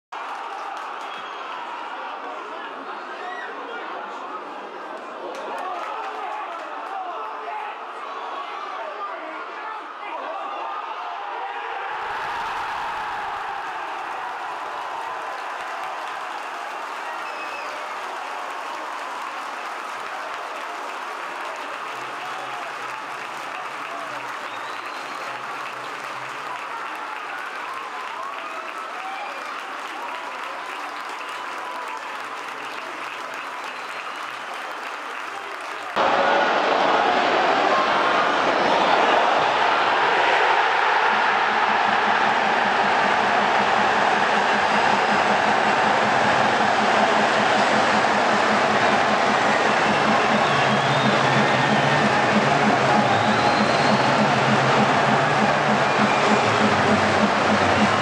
Goal - Tranmere v FC Halifax Town (18/8/15)
Andy Mangan's first goal for Tranmere seals victory over FC Halifax Town.